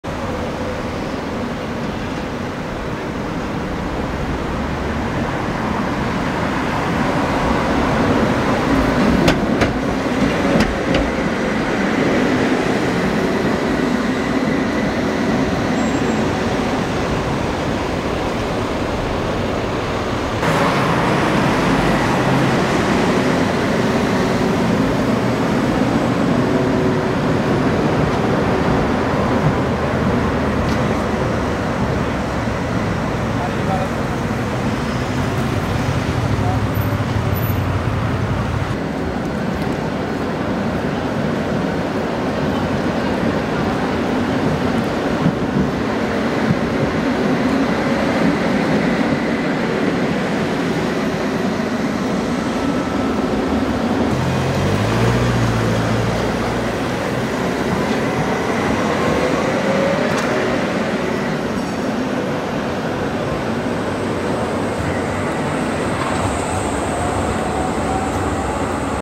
Nová tramvaj 8759 T3R.PLF na linkách 2 a 13, 15.-16.8.2024
Ve čtvrtek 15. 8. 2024 poprvé vyjela s cestujícími na linku tramvaj T3R.PLF 8759. Krátké zachycení provozu z prvních dvou dnů.